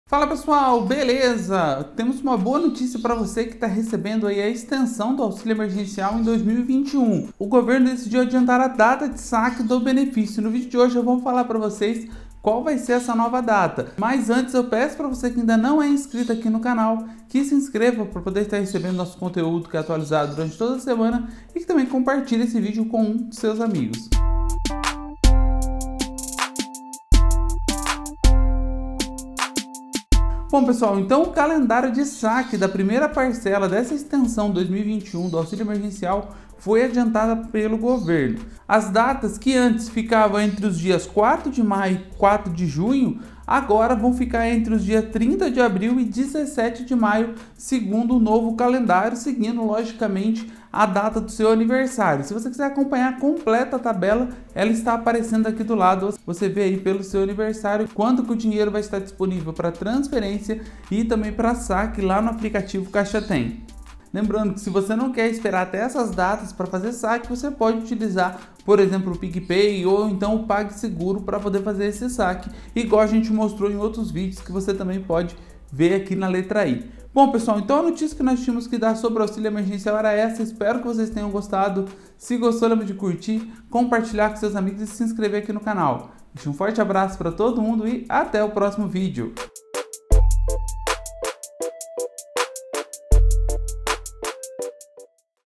GOVERNO ANTECIPA CALENDÁRIO DO AUXÍLIO EMERGENCIAL
A Caixa divulgou nesta semana o novo calendário para saque da primeira parcela da extensão 2021 do Auxílio Emergencial, confira no vídeo o que muda de acordo com o mês de seu nascimento.